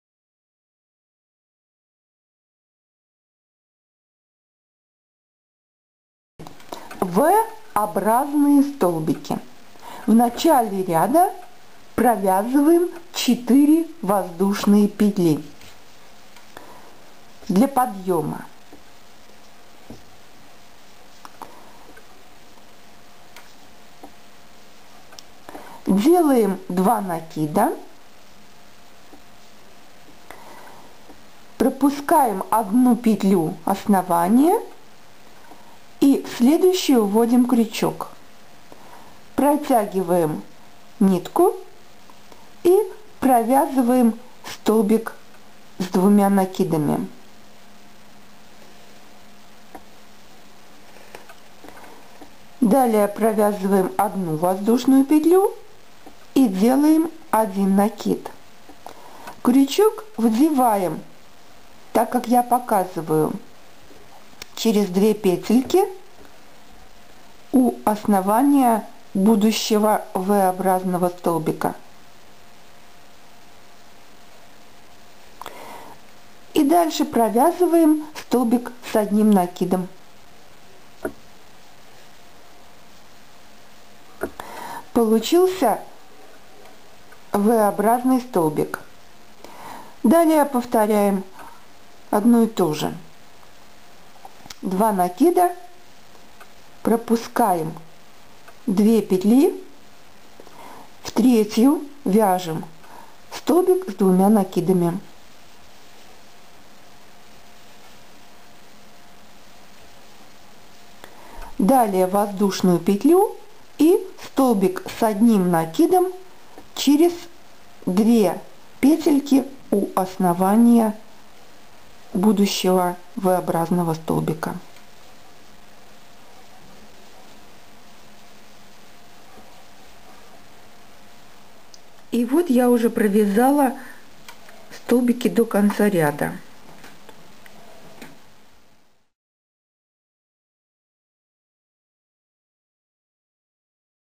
Вязание крючком - Урок 52 V-образный столбик
Сложные столбики. V-образный столбик. Урок вязания крючком для начинающих № 52